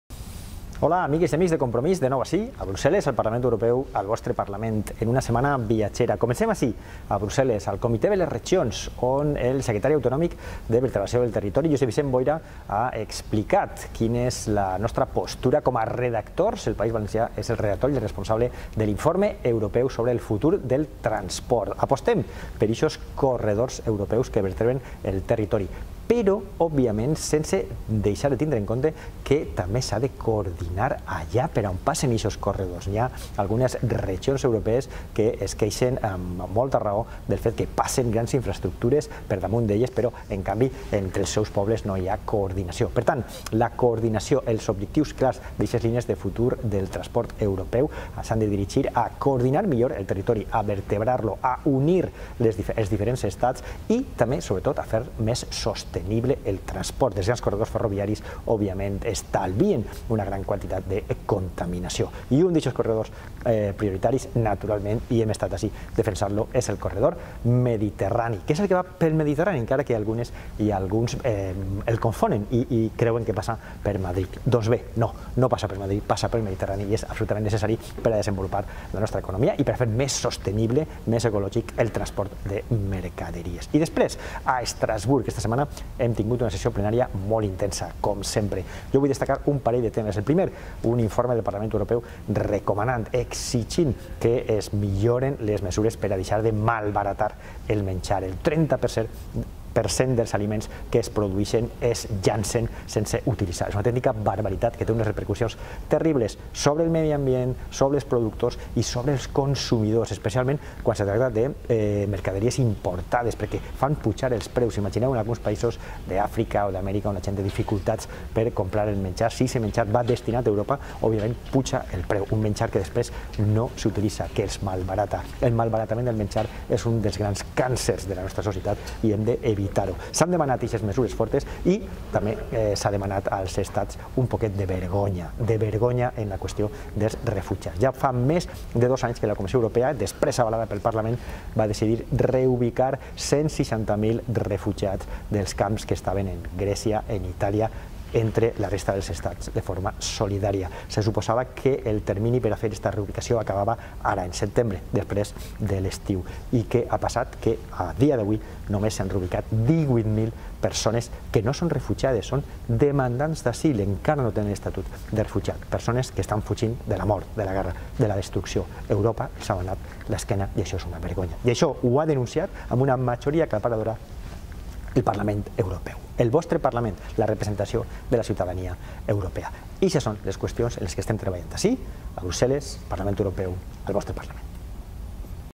"Corredor Mediterrani, malbaratament d'aliments, refugiats": videocrònica de Jordi Sebastià
El Portaveu de Compromís al Parlament Europeu, Jordi Sebastià, ens envia la crònica de l'activitat setmanal.